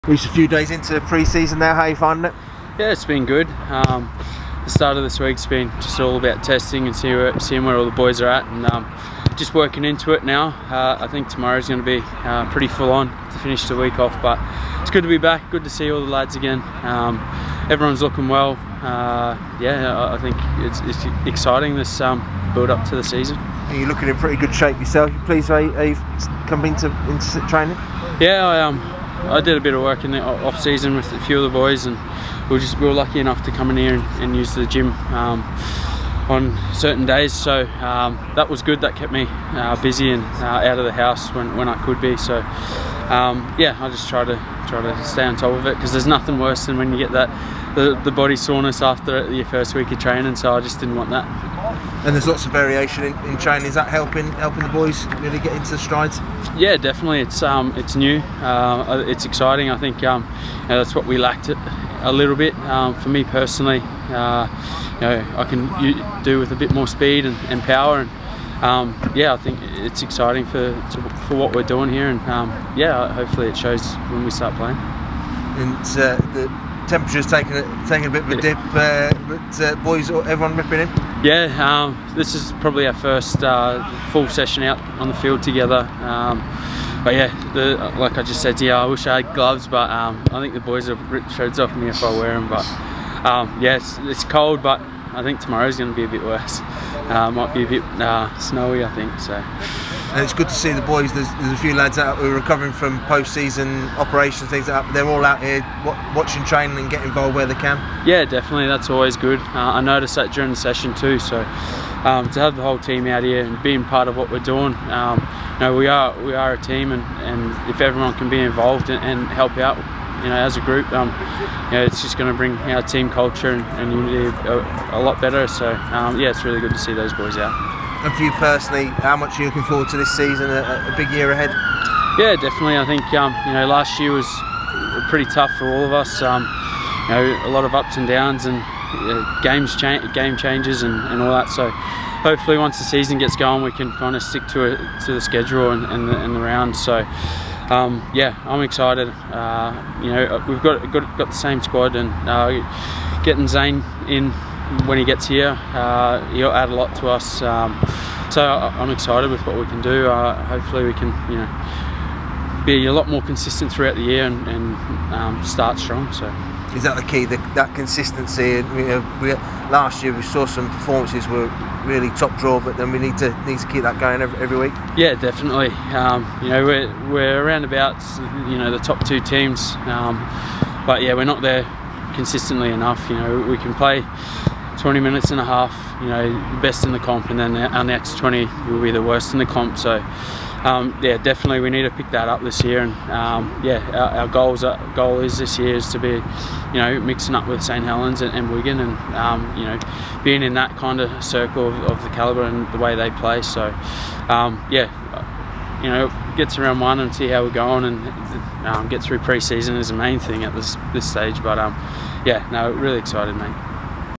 Martin excited by pre-season drive
Goal kicking ace Rhyse Martin says he is excited about the potential for this group of players to thrive on a full pre-season together despite the restrictions in place due to the global pandemic.

On a personal level, the PNG skipper says he has come into pre-season in good shape and looking to make the most of the new programmes for the squad this winter.